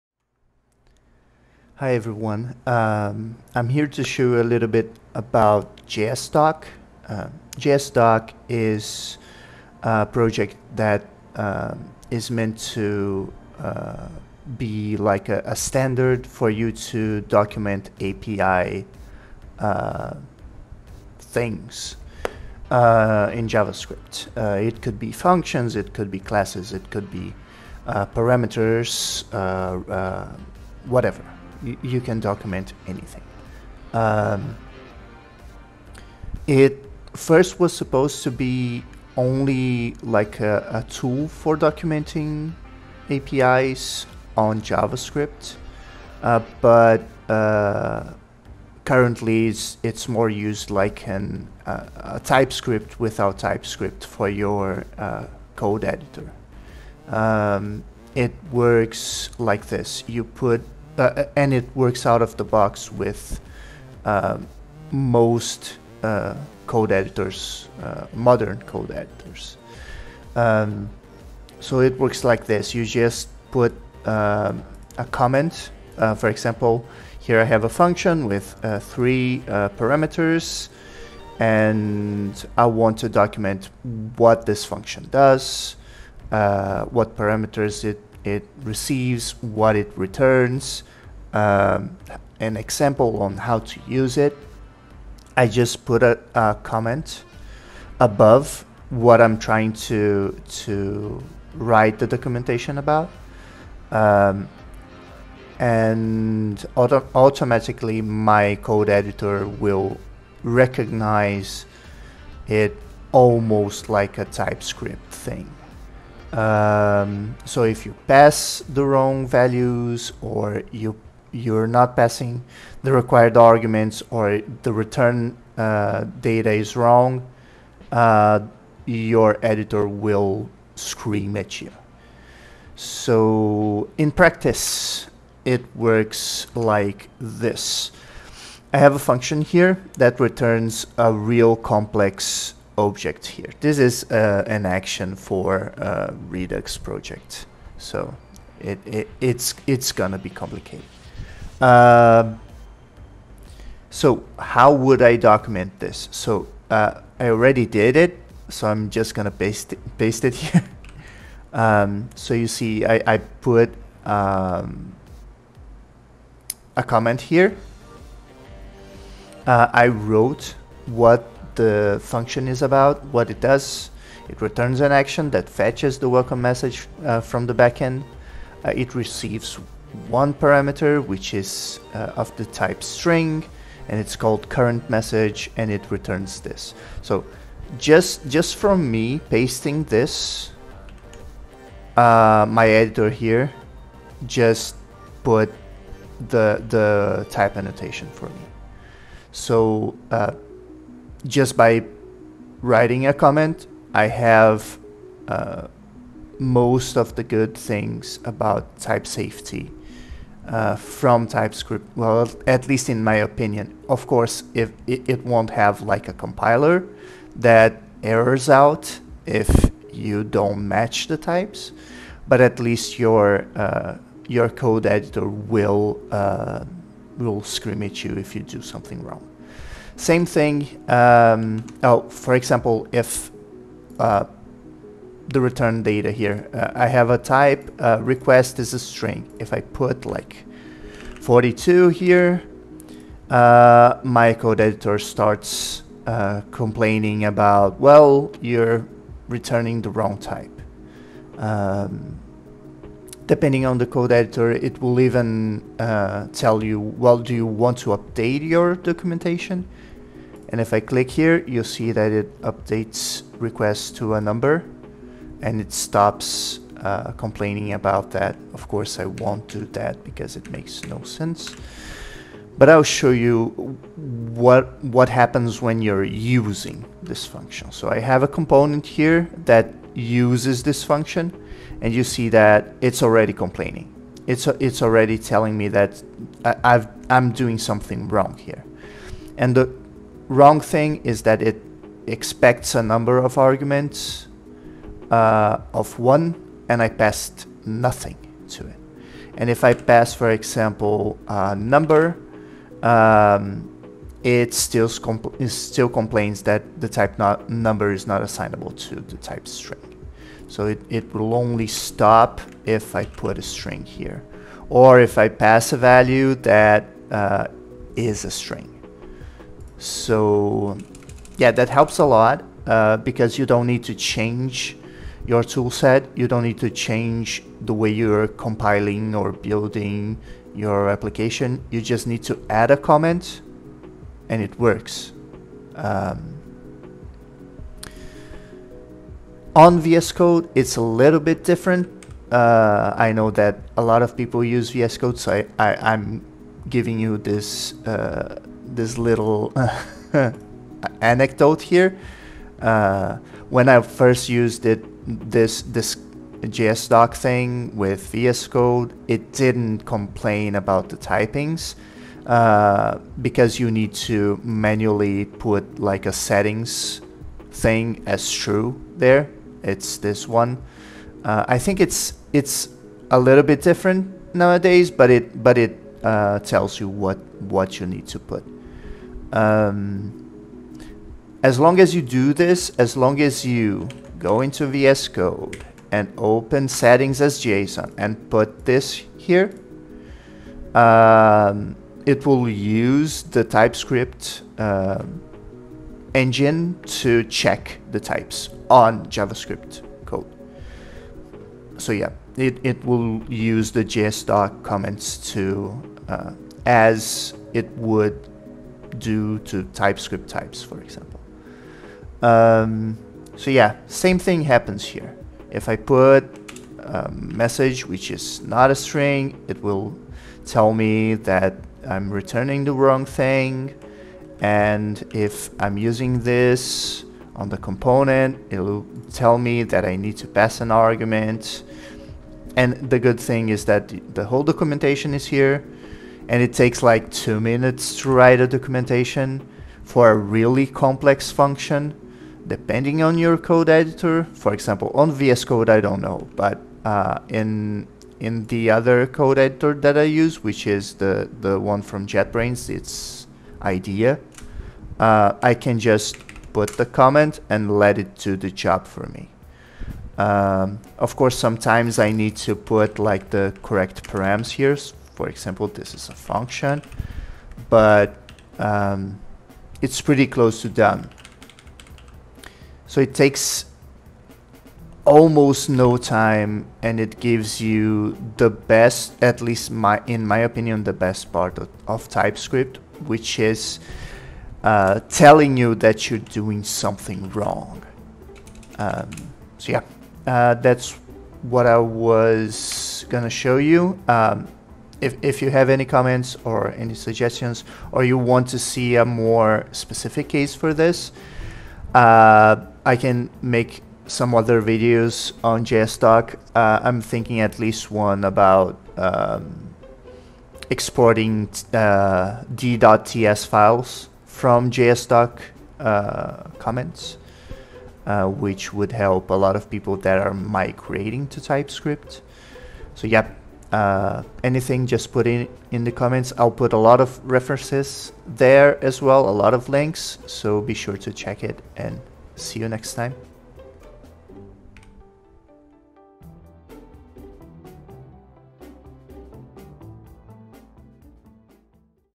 JSDoc - TypeScript without Typescript
JSDoc is a wonderful tool for those who want type checking on Javascript apps inside their text editor, but without migrating to TypeScript or needing to learn it.

It's really easy to use, fast to implement, and has a big impact on the day-to-day basis of a Javascript project. All of that, without needing to install anything else.Ele é fácil de usar, tem um custo muito baixo de tempo e tem um impacto bem grande no dia-a-dia de um projeto javascript. Tudo isso, sem ter que instalar nada.Ele é fácil de usar, tem um custo muito baixo de tempo e tem um impacto bem grande no dia-a-dia de um projeto javascript. Tudo isso, sem ter que instalar nada.